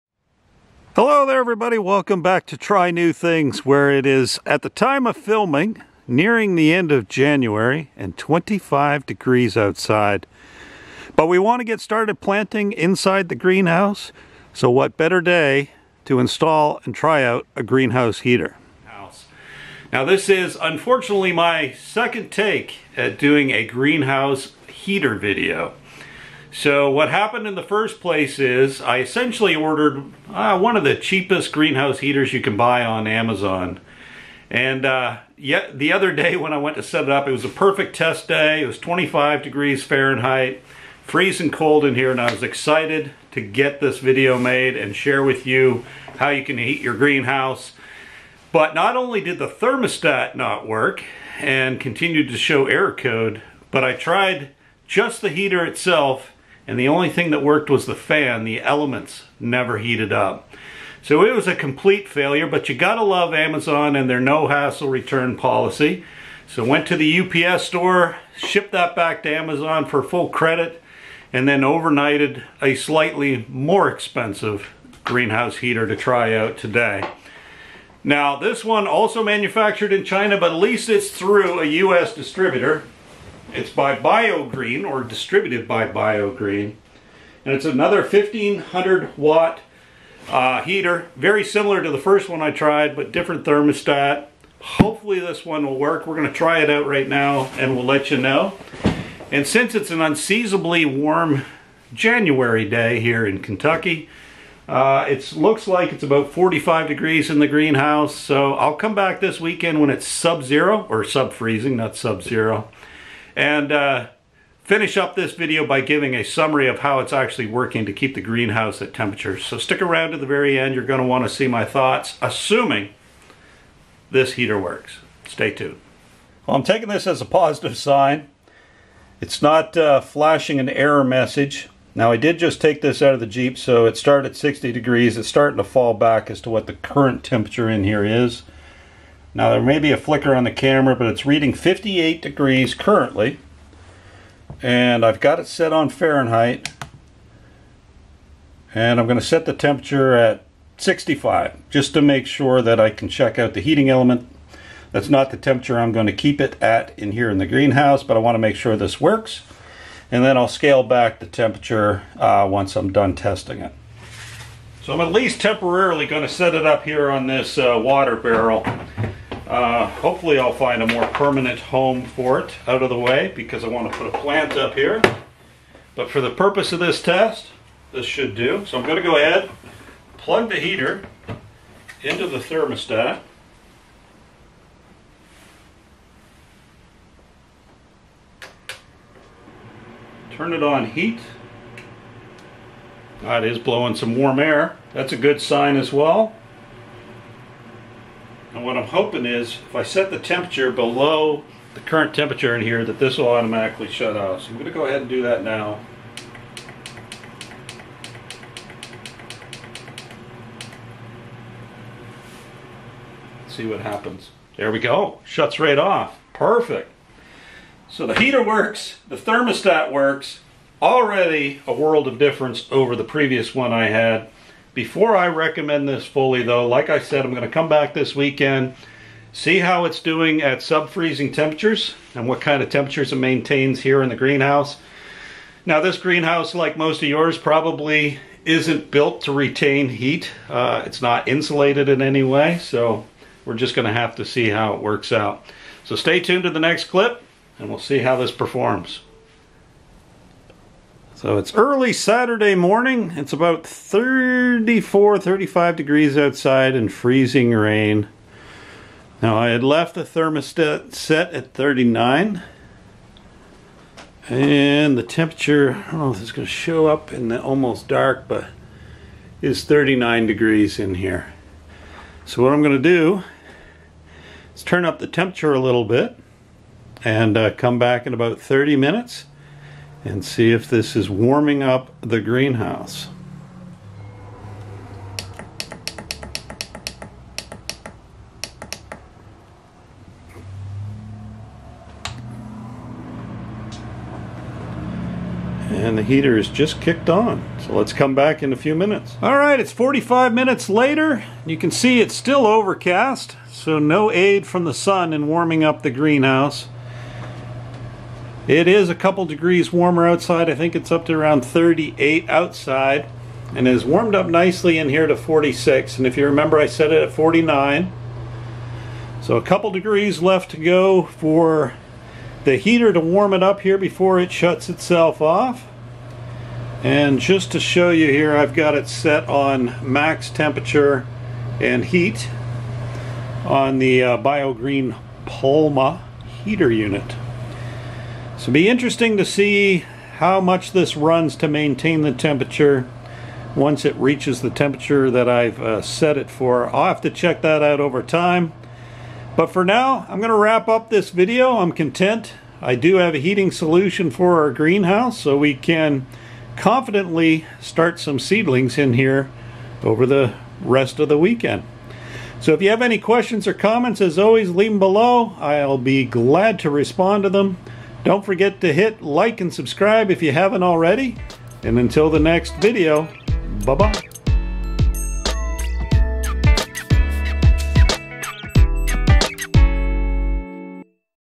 TNT #163: Greenhouse 120v Bio Green Electric Heater Review - 1500 w / 5120 btu
In this episode, I unbox and review a 1500w Electric Greenhouse Heater from Bio Green.